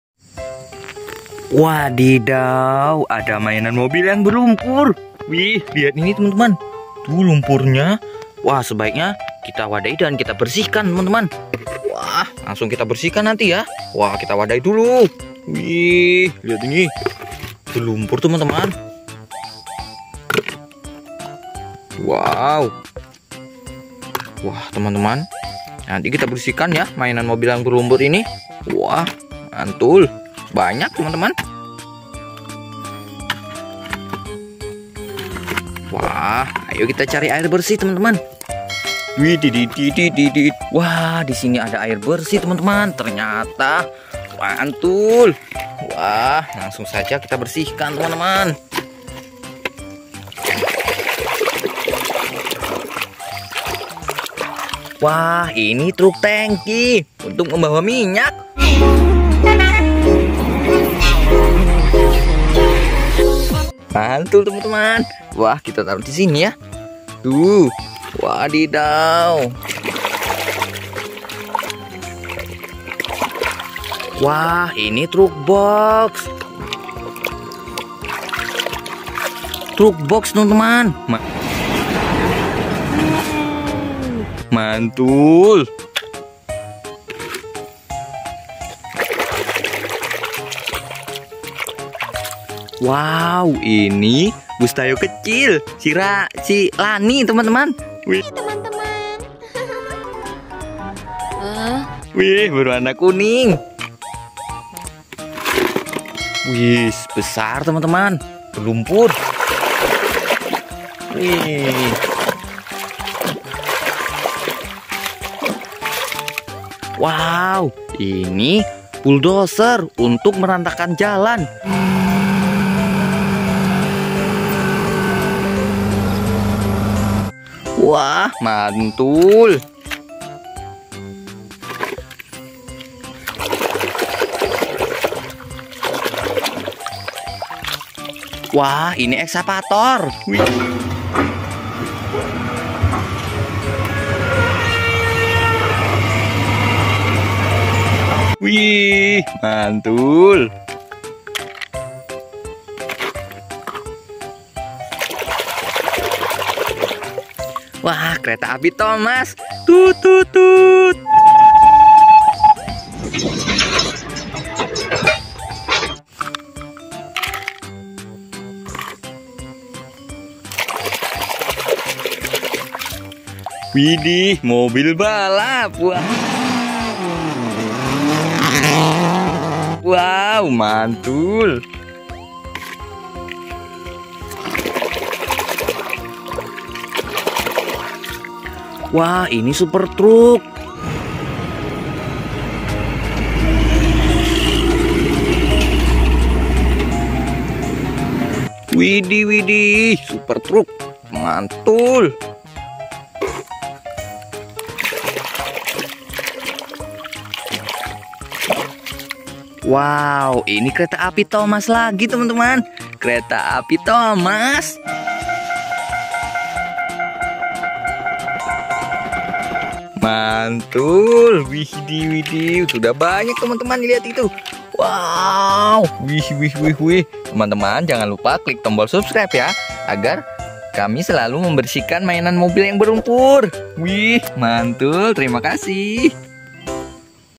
Membersihkan Mainan Mobil Yang Berlumpur, Bull Dozer, Excavator, Truk Panjang, Truk Tangki
Membersihkan Mainan Mobil Yang Berlumpur, Bull Dozer,  Excavator, Truk Panjang, Truk Tangki, Truk Box, Kereta Api Thomas, Tayo, Mobil Balap